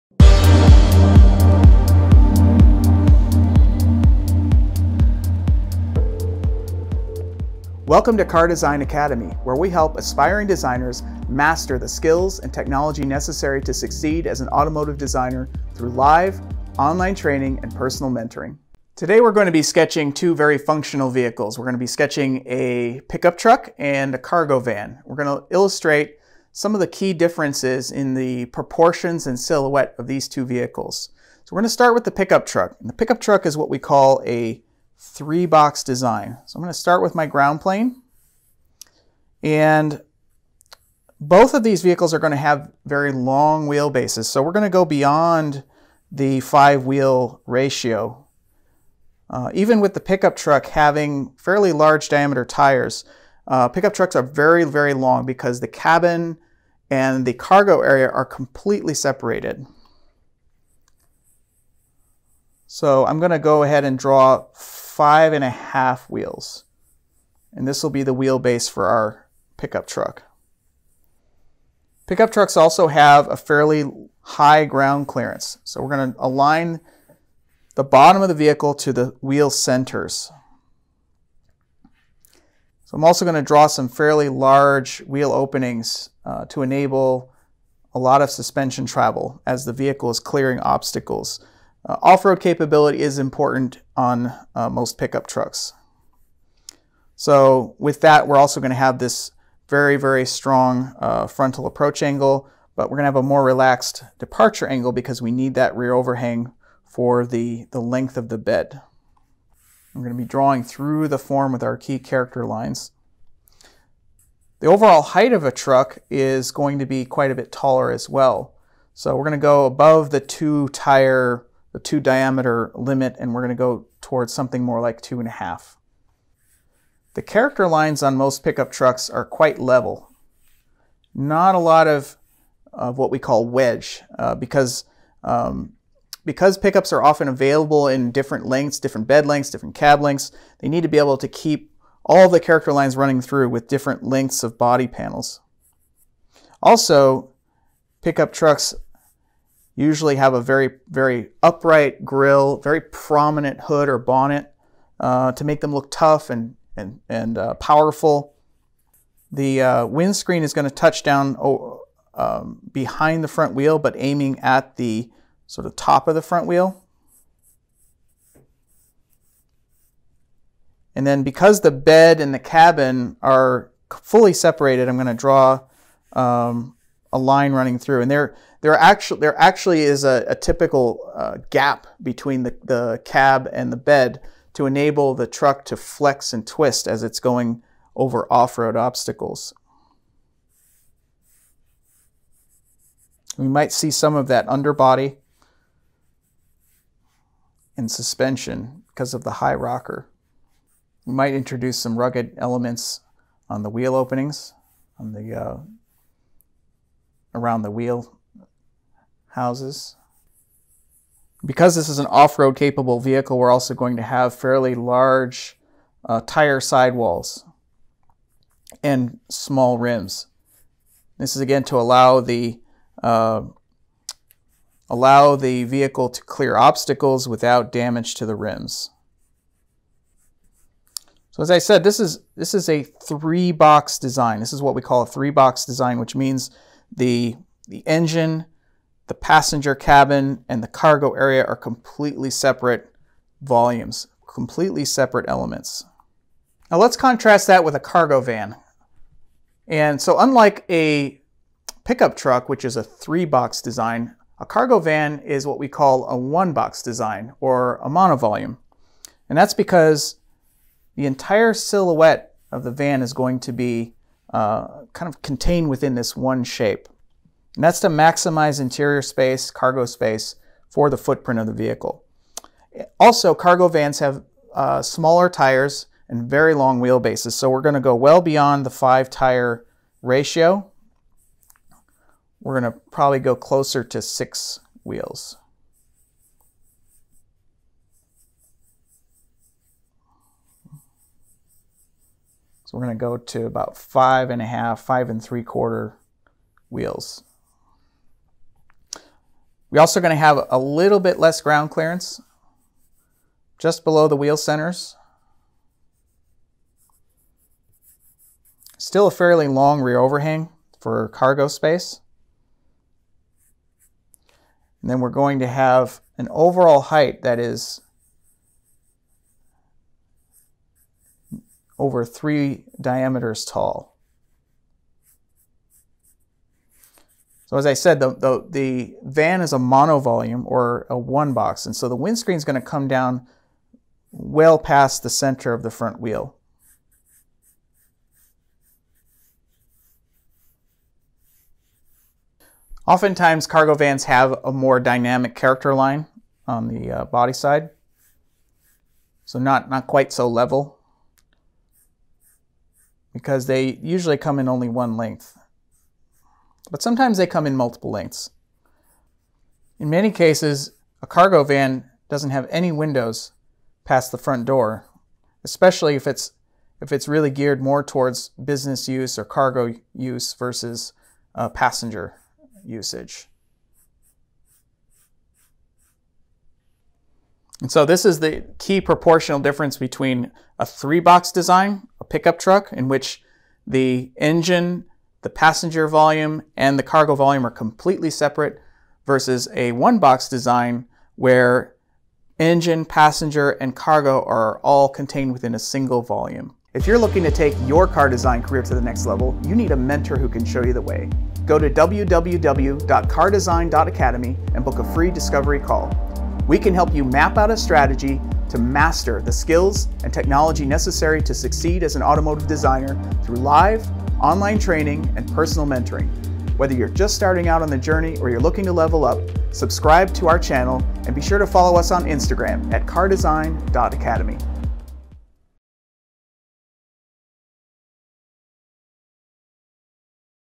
Car Design 101: Sketching Trucks & Vans in Side View
Schedule a free discovery call to learn more about our live, online training and personal mentoring!

In this tutorial we'll learn how to sketch a pickup truck and a cargo van in side view.